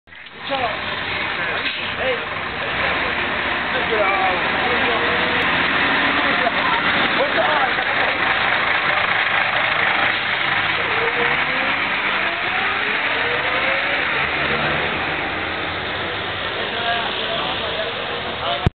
Odjezd autobusu Ikarus 280 na lince 30 v Budapešti
Odjezd maďarského autobusu Ikarus 280 na lince 30 v Budapešti. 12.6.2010